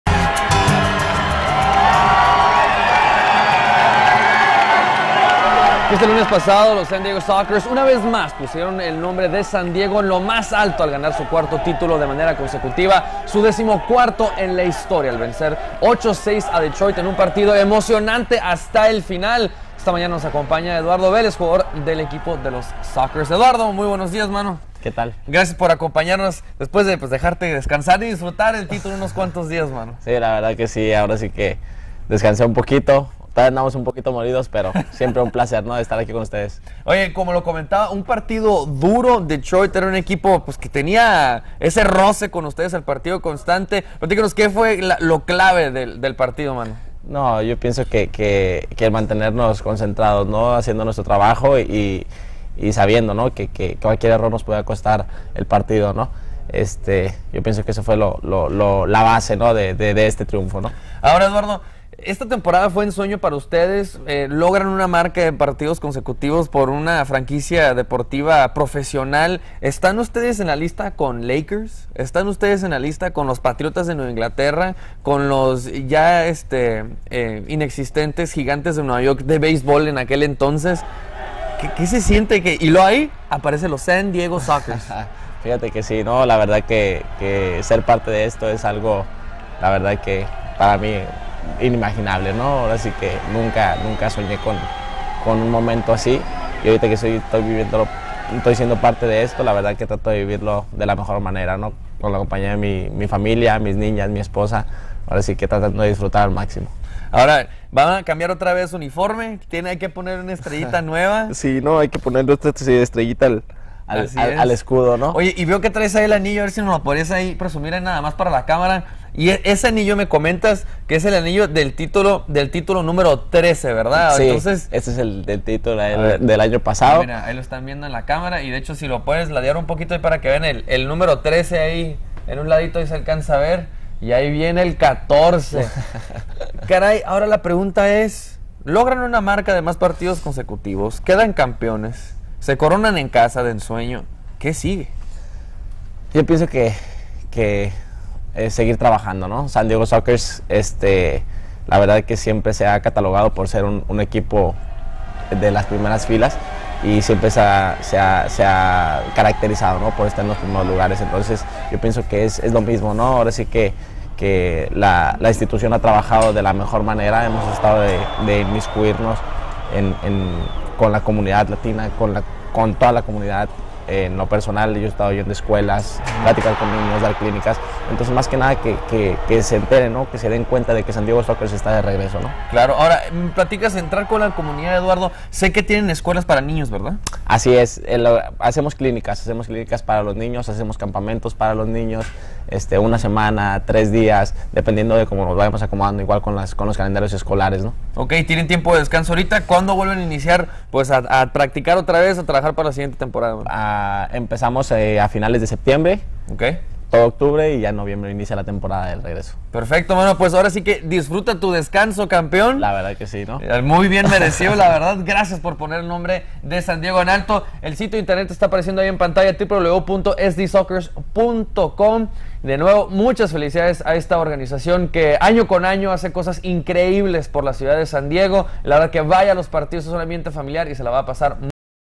SAN DIEGO SOCKERS
EDUARDO VELEZ JUGADOR DE SAN DIEGO SOCKERS NOS CUENTA SU EXPERIENCA EN ESTE CAMPEONATO QUE SE CORONO POR 4 OCASION CONSECUTIVA